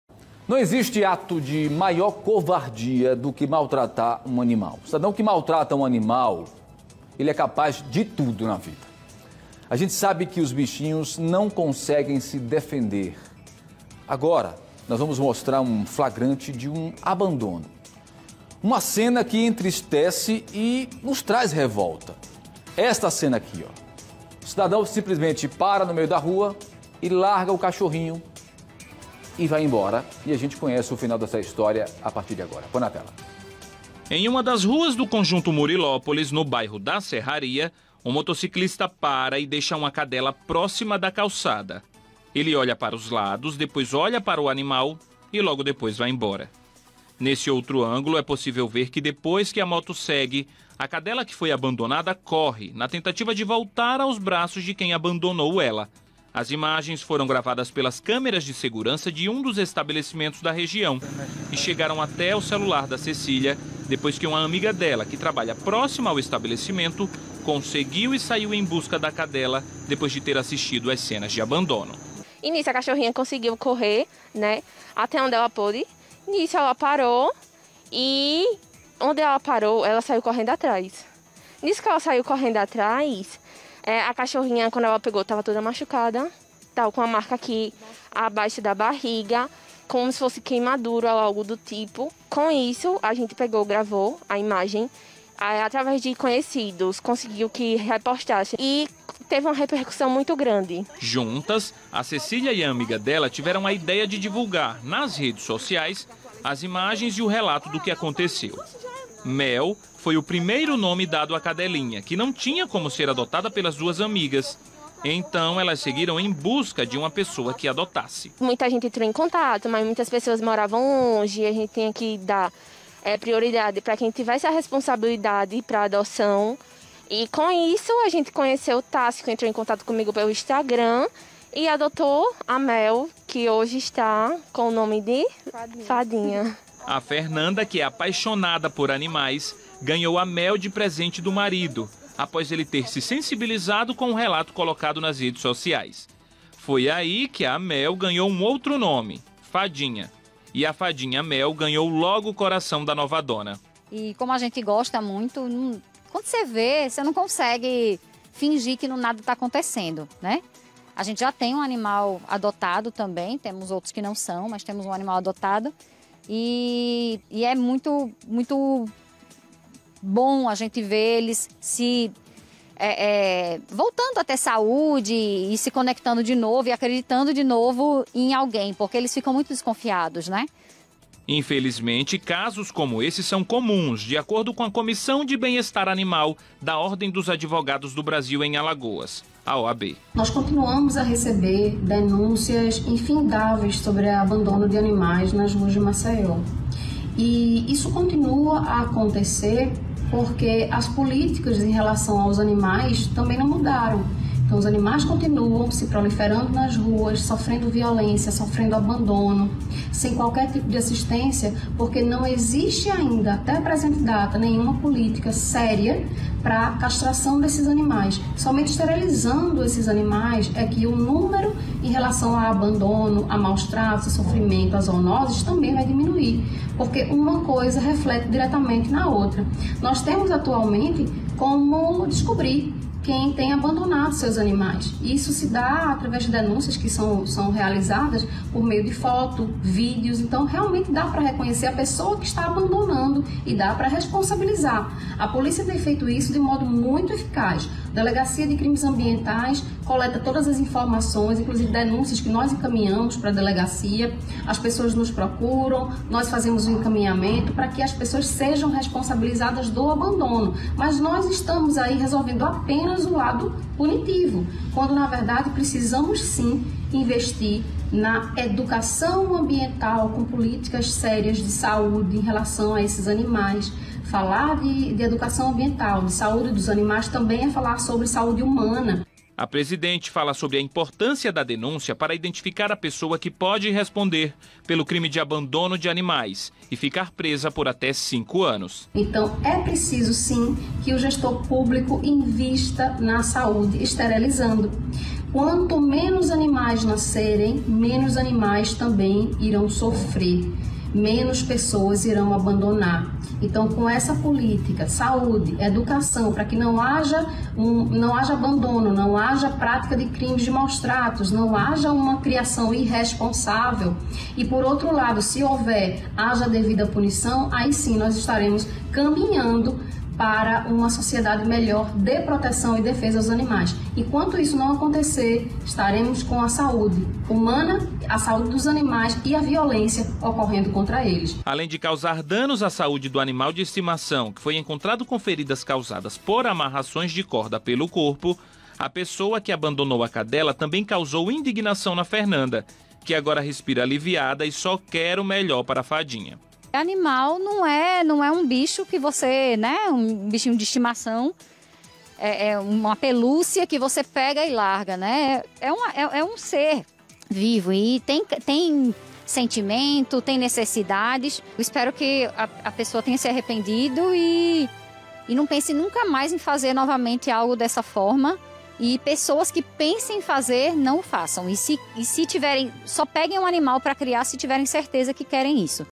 Covardia: Homem abandona cachorrinha e o animal corre atrás da moto depois de ter sido largado
Acompanhe detalhes sobre a covardia, na reportagem de Netto Motta.